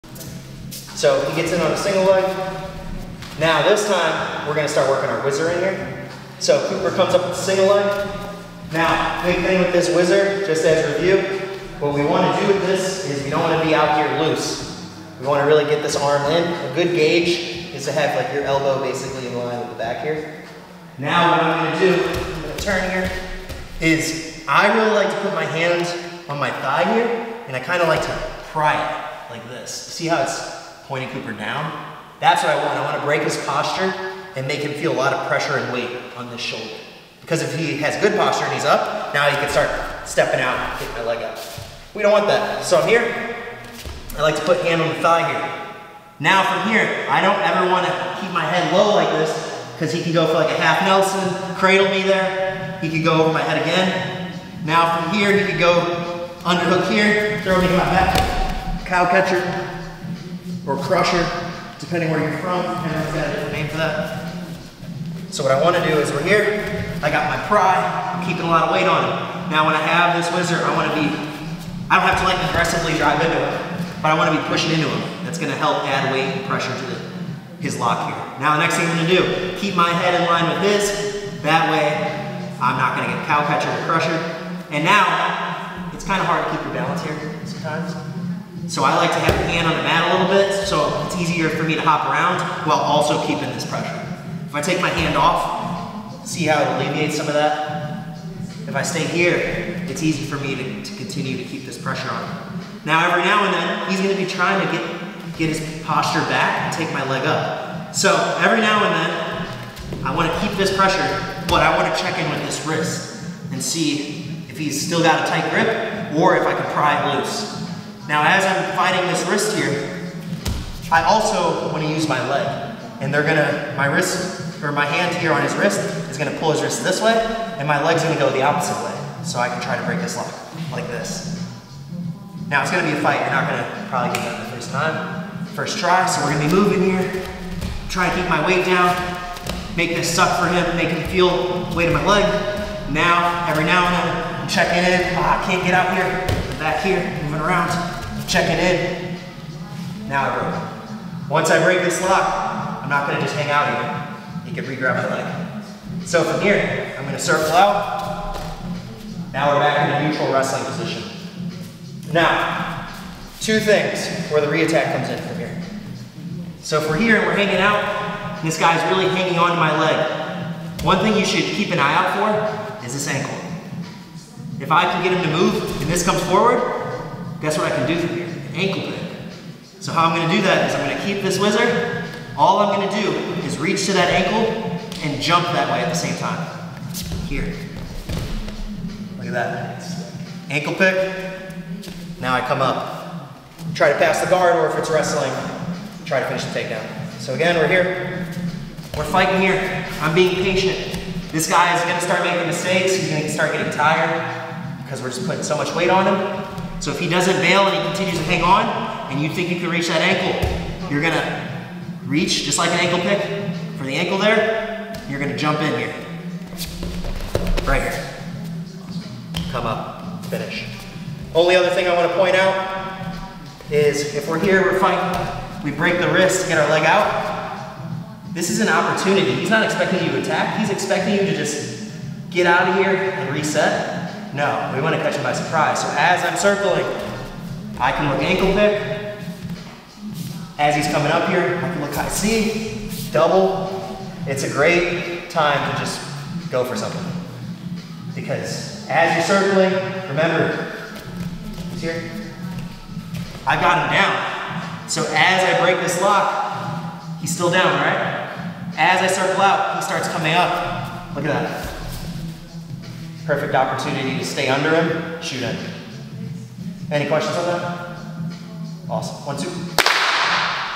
Turn your Defense into Offense with a Whizzer
This technique video covers
- How to do the whizzer
- How to use it to get to an ankle pick
- How to get your leg back and reattack

*Email List (Unsubscribe Anytime)*